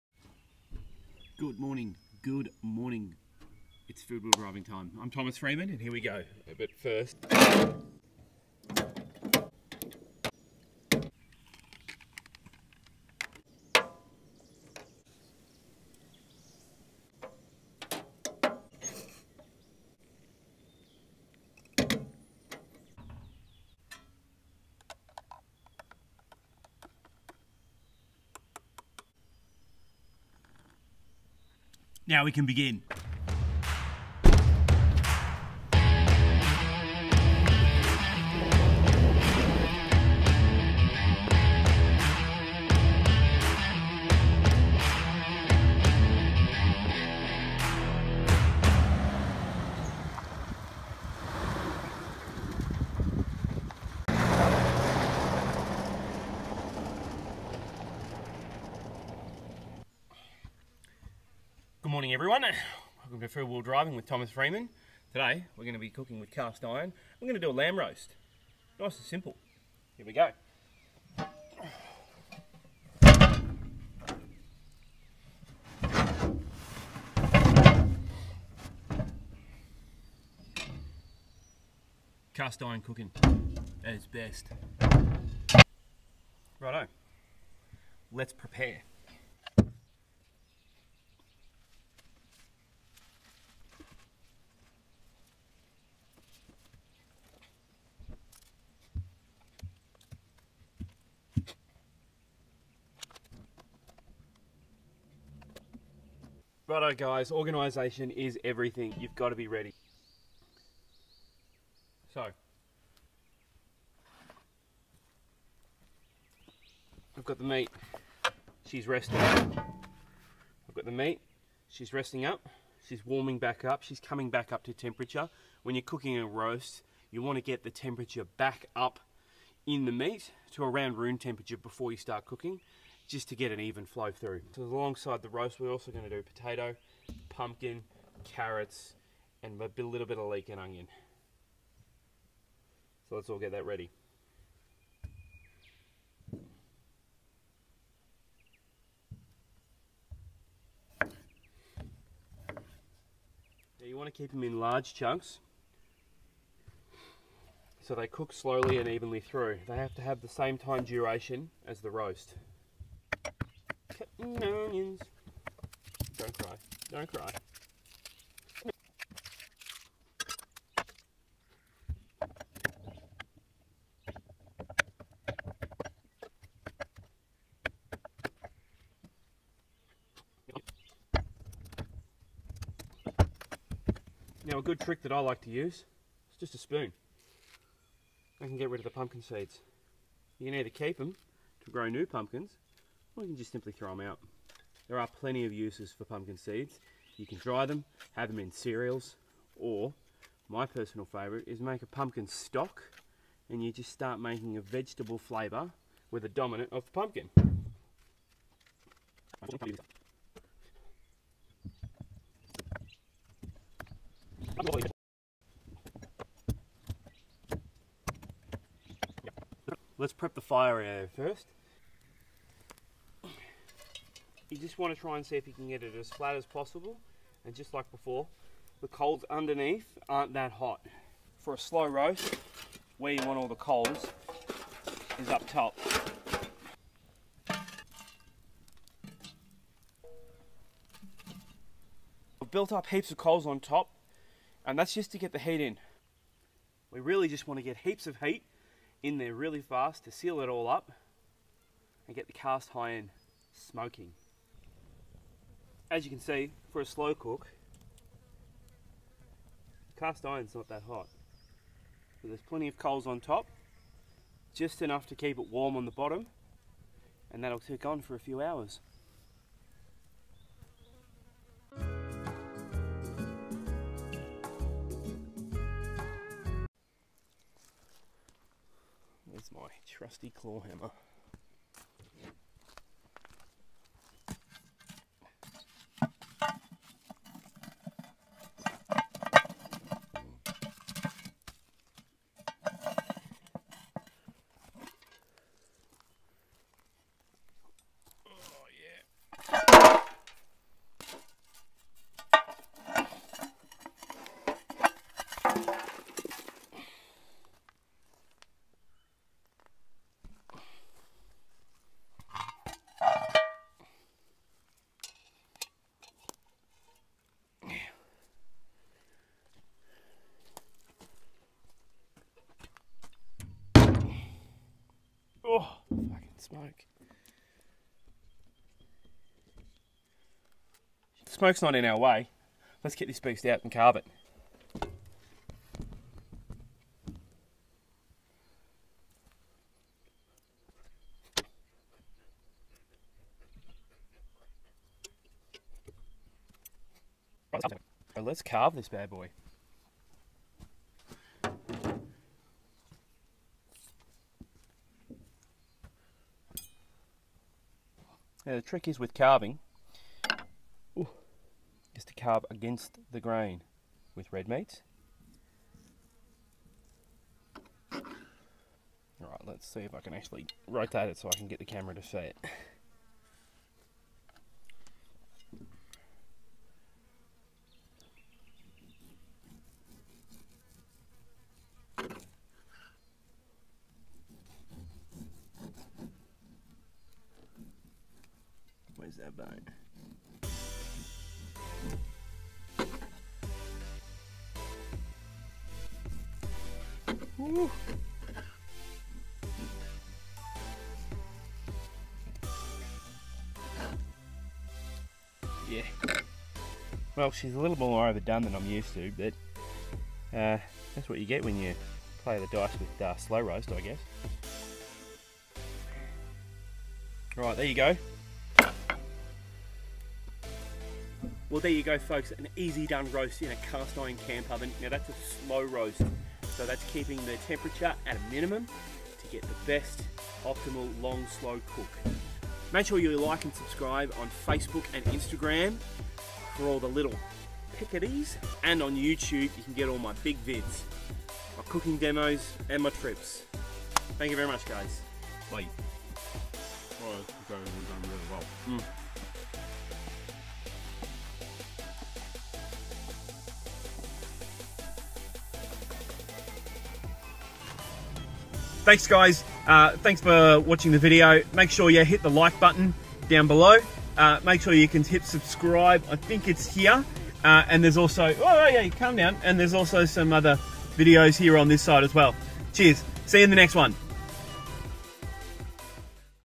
Camp Oven Roast - Cast Iron Cooking
Lets Get into it, we are back to basics this month with a back to back vibe, sit back for Easy Cooking! hopefully you learn some techniques.